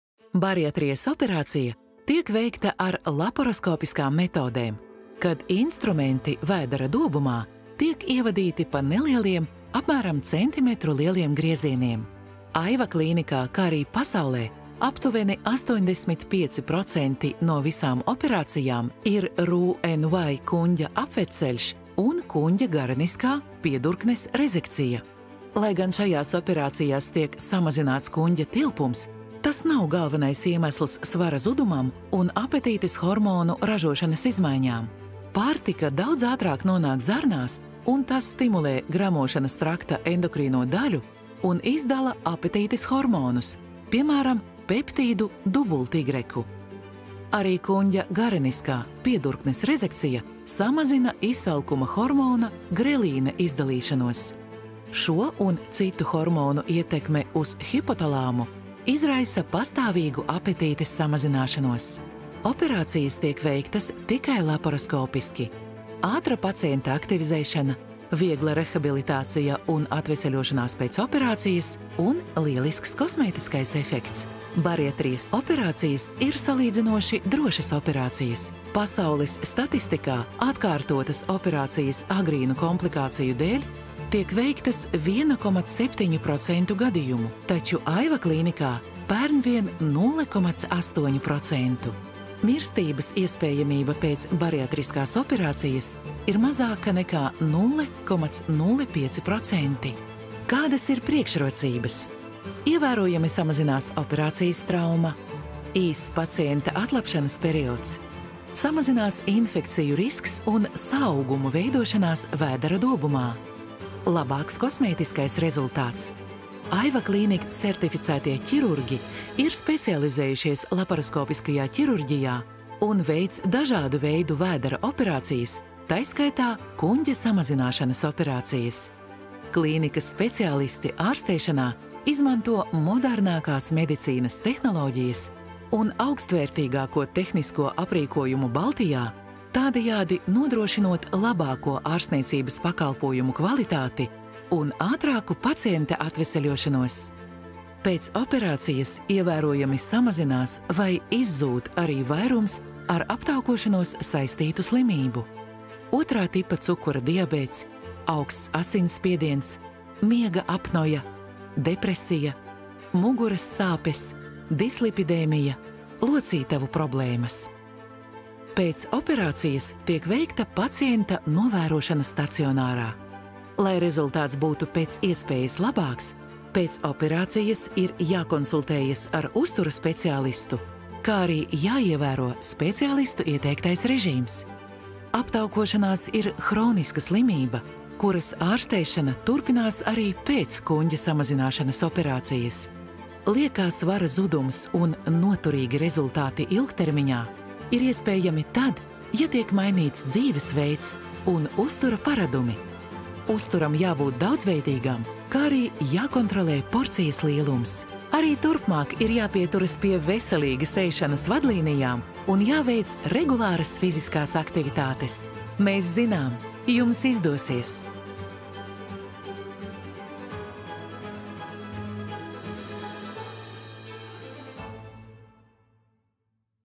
Bariatrijas operācija ir kuņģa samazināšanas vai pārveidošanas operācija
Bariatrijas operācija ir kuņģa samazināšanas vai pārveidošanas operācija, kas palīdz izmainīt organismā notiekošos barības apstrādes procesus.